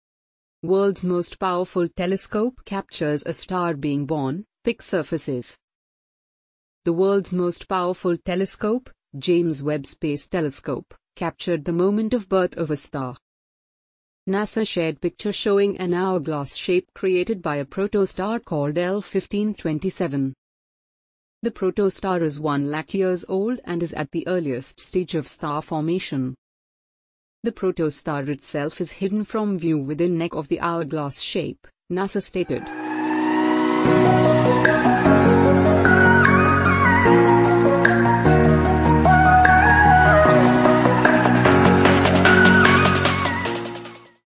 World's most powerful telescope captures a star being born, pic surfaces
The world's most powerful telescope, James Webb Space Telescope, captured the moment of birth of a star. NASA shared picture showing an hourglass shape created by a protostar called L1527. The protostar is 1,00,000 years old and is at the earliest stage of star formation. The protostar itself is hidden from view within "neck" of the hourglass shape, NASA stated.